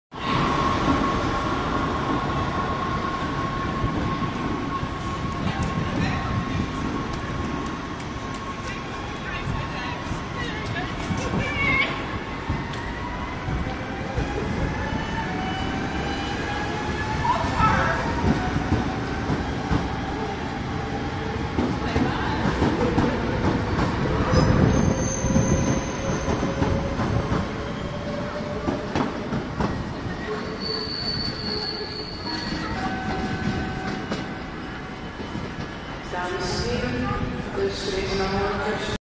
NS VIRM 9559 vertrekt uit Leiden Centraal
De NS VIRM 9559 vertrekt uit Leiden Centraal als IC richting Lelystad Centrum. Bedankt voor het kijken!